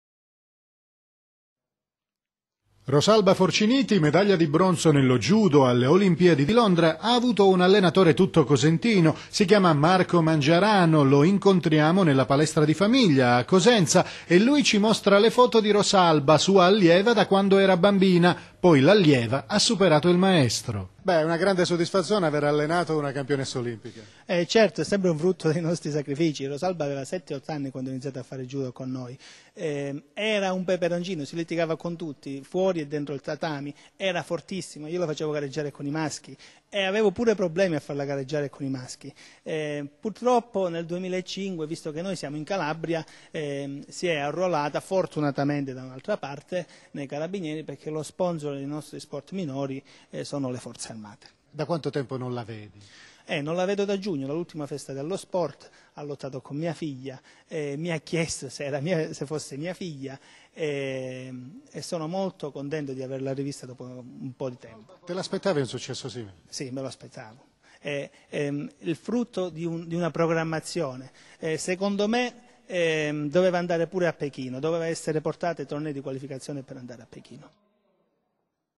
Cosenza: Marco Mangiarano, l'allenatore di Rosalba Forciniti
Rosalba Forciniti, medaglia di bronzo nello judo alle Olimpiadi di Londra, ha avuto un allenatore tutto cosentino. Si chiama Marco Mangiarano. Lo incontriamo nella palestra di famiglia, a Cosenza. E lui ci mostra le foto di Rosalba, sua allieva da quando era bambina. Poi l'allieva ha superato il maestro.